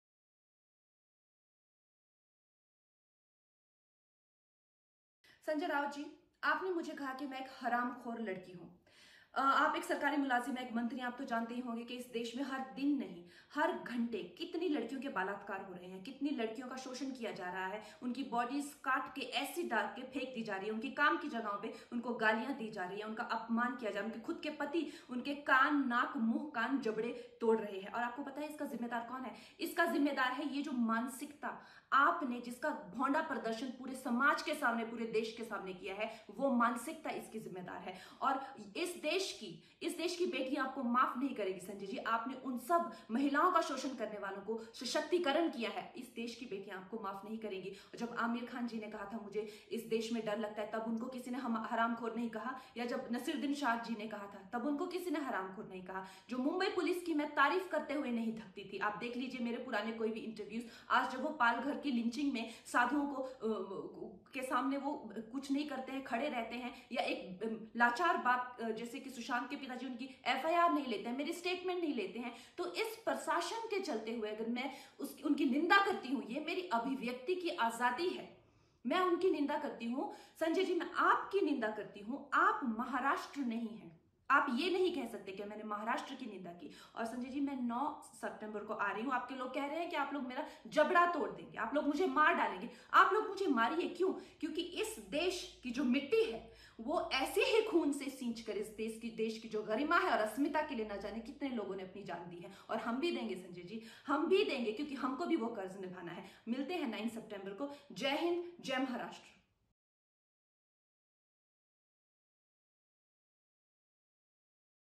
‘Kill me, lets’ meet on Sep 09’ Kangana to Sanjay Raut
Manali (Himachal Pradesh), Sep 06 (ANI): Bollywood actress Kangana Ranaut hit back on  Shiv Sena MP Sanjay Raut’s over his "Haramkhor Ladki" remark. Kangana said, “Sanjay Raut encourages women abusers, daughters of India will not forgive you. Your people are saying they want to kill me, you want to kill me? then do it, let’s meet on September 09, jai Hind, Jai Maharashtra .”